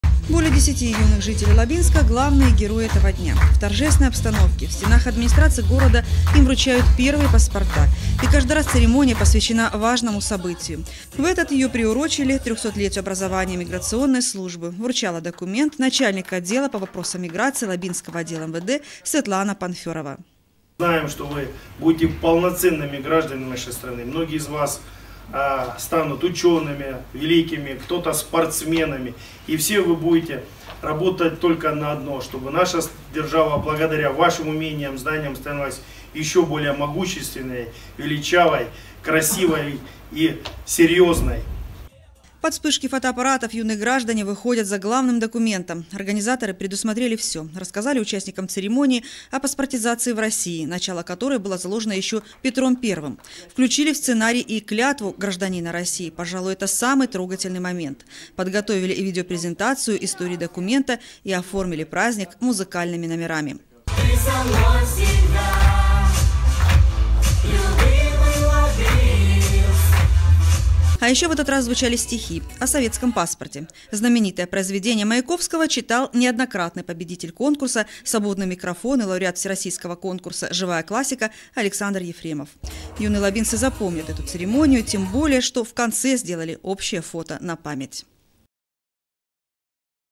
Вручение паспортов.
Юбилею миграционной службы посвящается. В администрации города  состоялась церемония торжественного вручения паспортов.